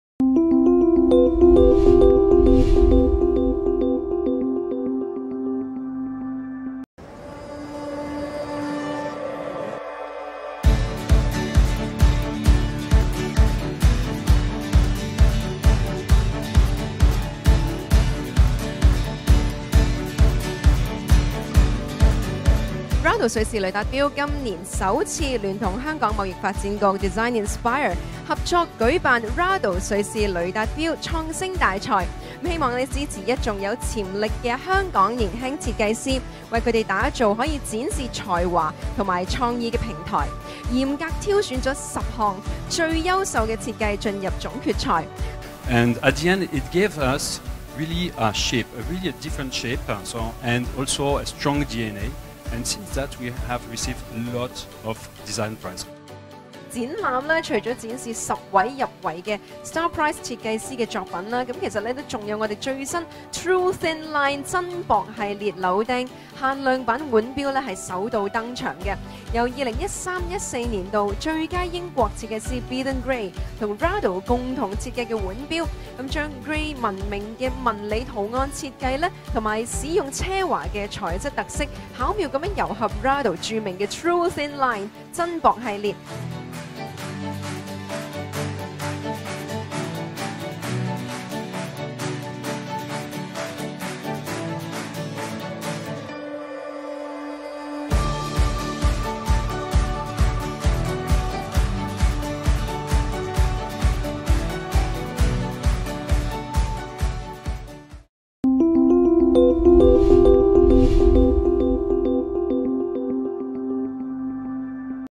The Rado Star Prize Hong Kong 2018
Rado's Star Prize Hong Kong edition focuses on the concept of "Design Inspired by Nature" - a theme hoping to explore the influence that nature has on design.  The Rado Star Prize, which is held internationally to promote the work of young designers, is another example of Rado’s first collaboration with DesignInspire 2018, an international exhibition about creativity organised by Hong Kong Trade Development Council.

Official Authorised Retailers for Rado.